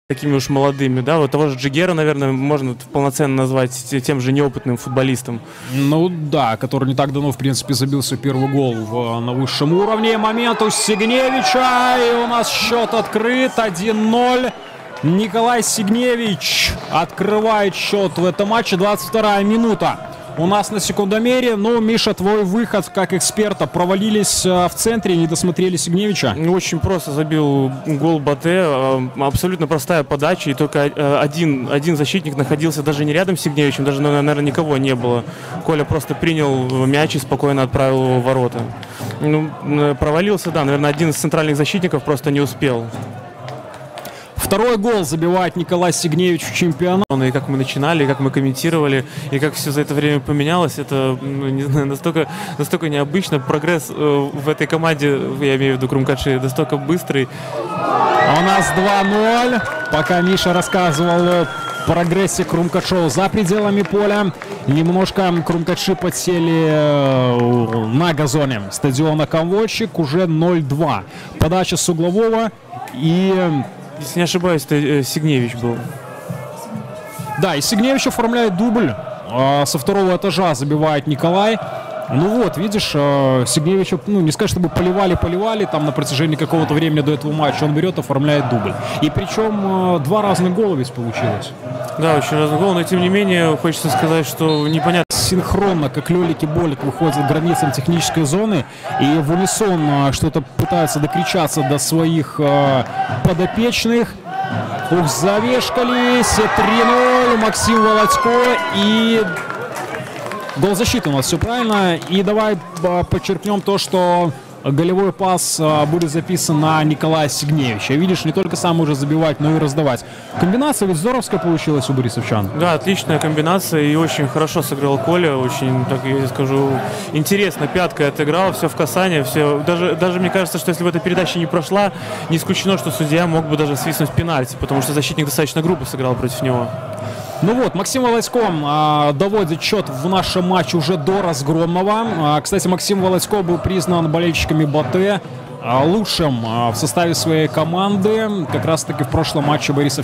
"Крумкачы" - БАТЭ - 0:3
В Минске на стадионе «Минск» программу 27-го тура открыл матч «Крумкачы» — БАТЭ, в котором борисовский клуб одержал крупную победу.